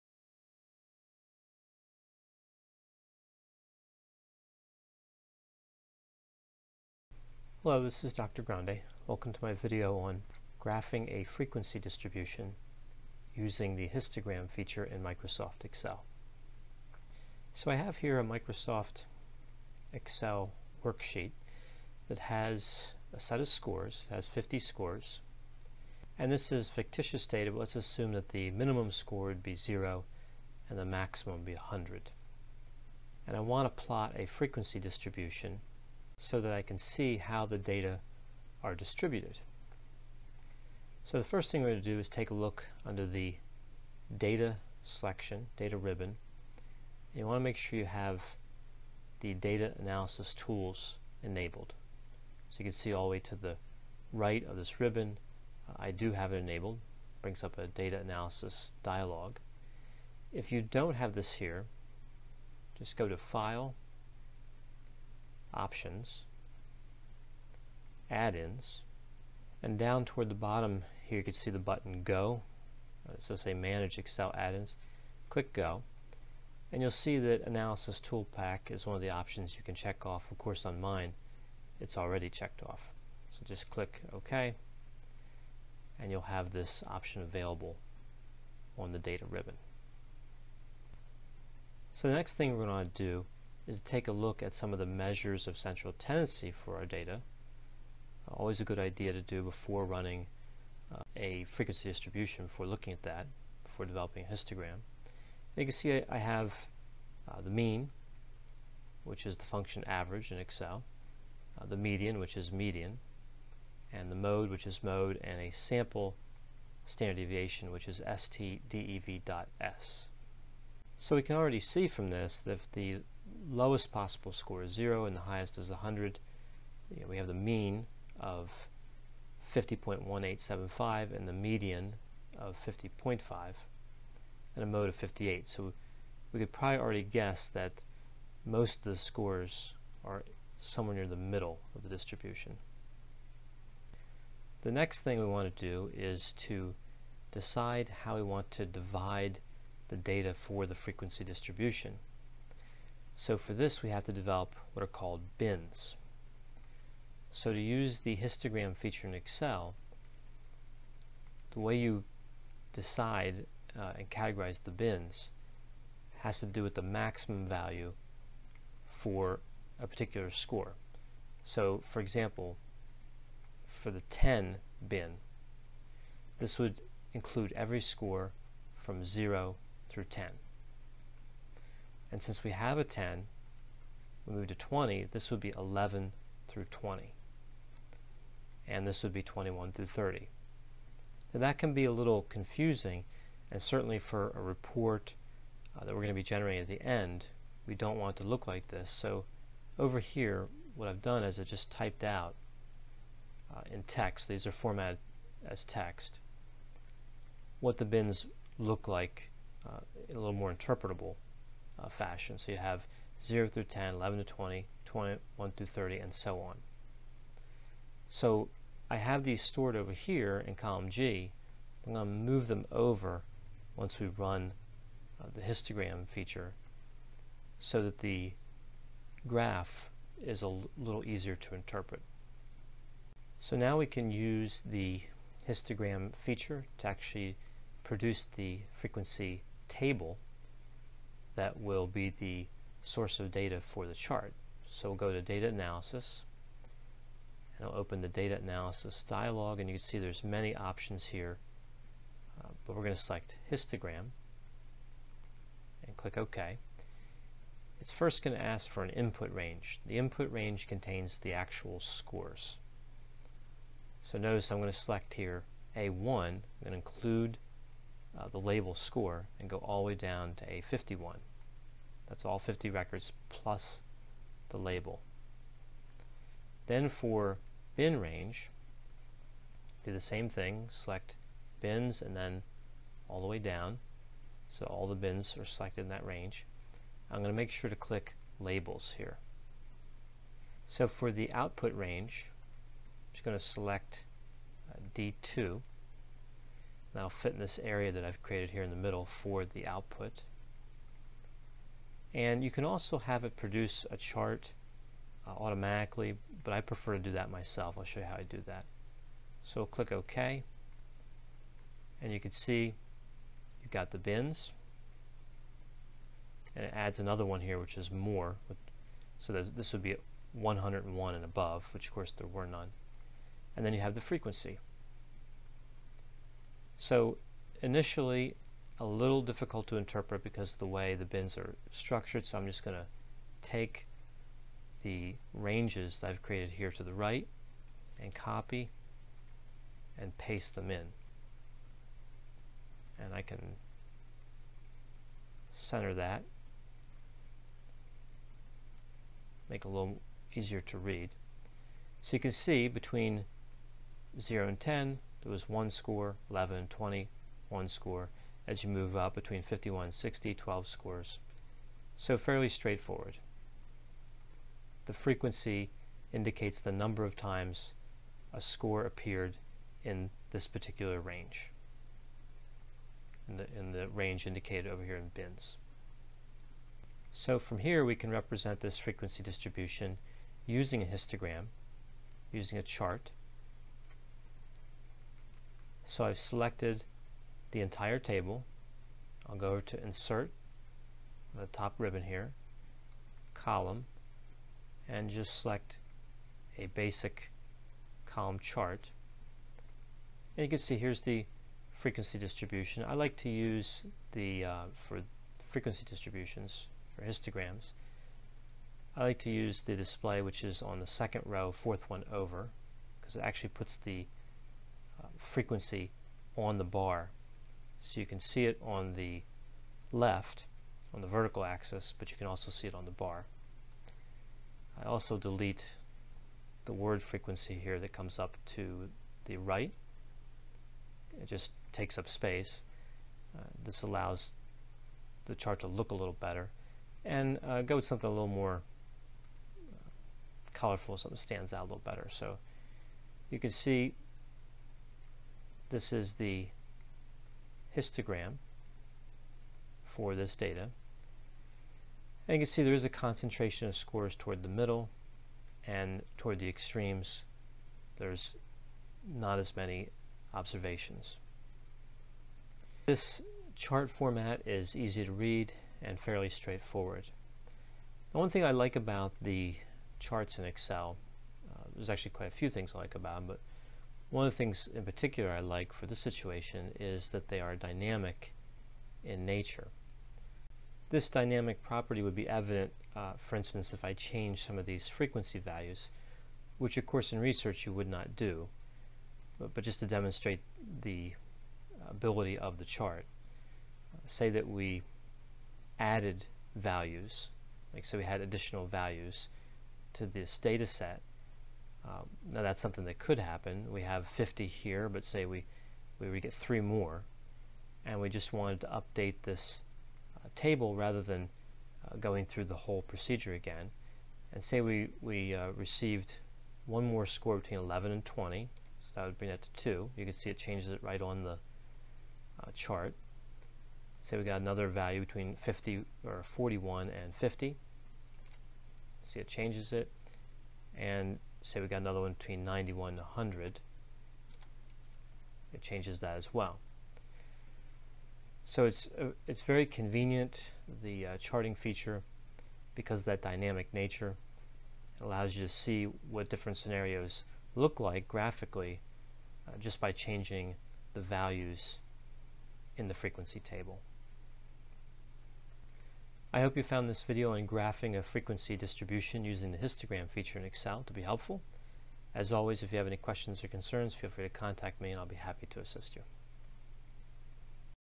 Graphing a Frequency Distribution in Excel Using the "Histogram" Feature
This video demonstrates how to graph a frequency distribution using the “Histogram” feature in Microsoft Excel.